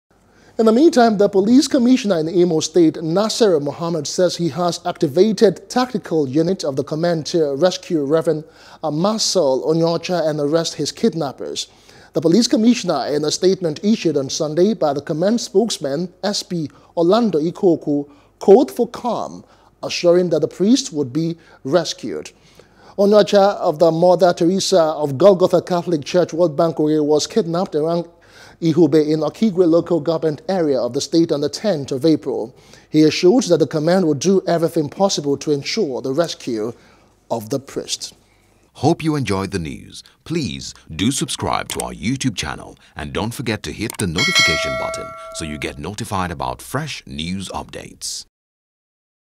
Police Begin Manhunt For Kidnappers Of Imo Priest | NEWS
The Police Commissioner in Imo, Nassiru Mohammed, says he has activated tactical units of the command to rescue Rev. Marcel Onyeocha and arrest his kidnappers.
The police commissioner, in a statement issued on Sunday by the command’s Spokesman, SP Orlando Ikeokwu, called for calm, assuring that the priest would be rescued.